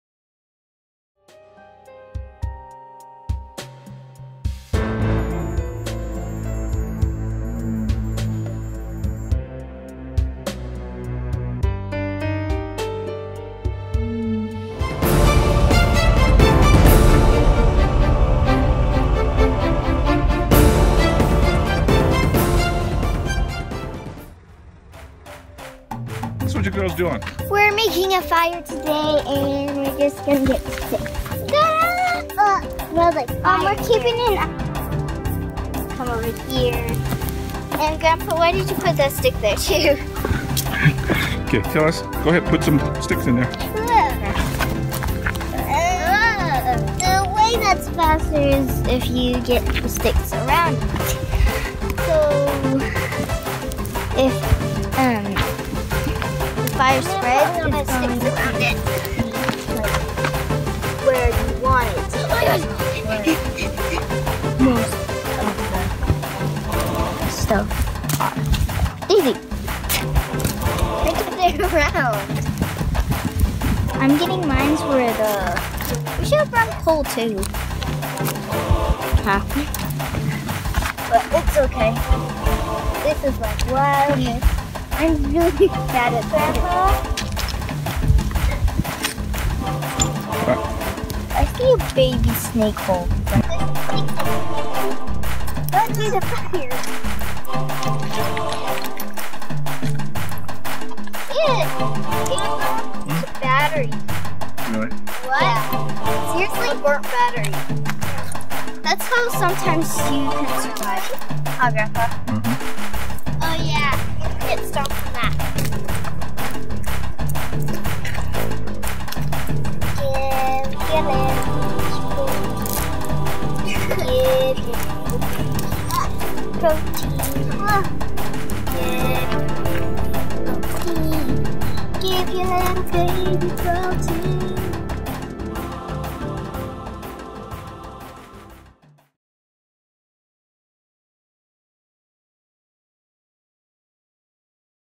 Adventure Friday With Grandpa
Friday adventure enjoying the mountain with the girls.